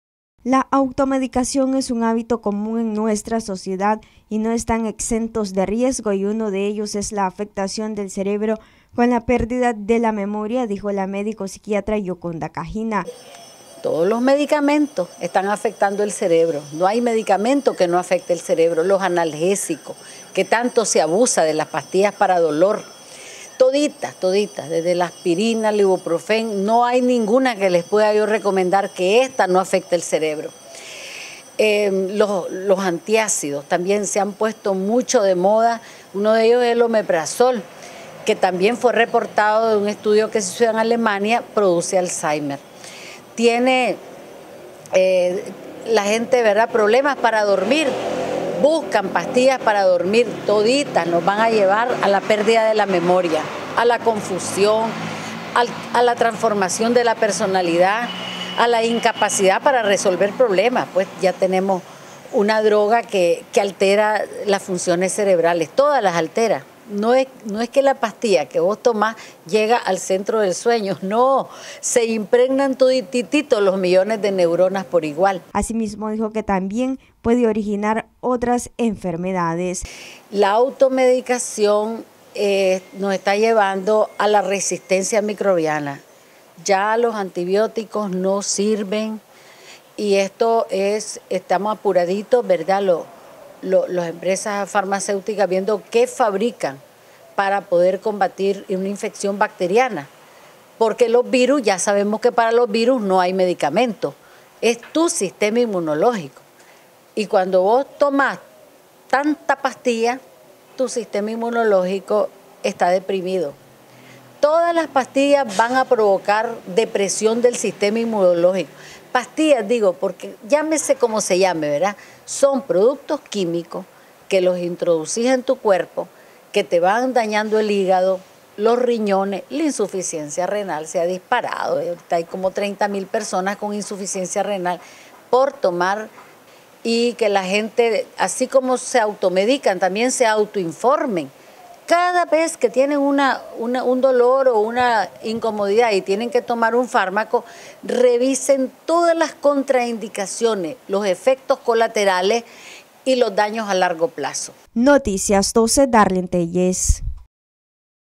Automedicación en las personas, ocasiona diversas enfermedades y pérdida de la memoria
Noticias12Nicaragua Gioconda Cajina Médico Psiquiatra, dijo que es preocupante que muchas personas continúan automedicándose, sin medir las consecuencias de enfermedades que pueda causar.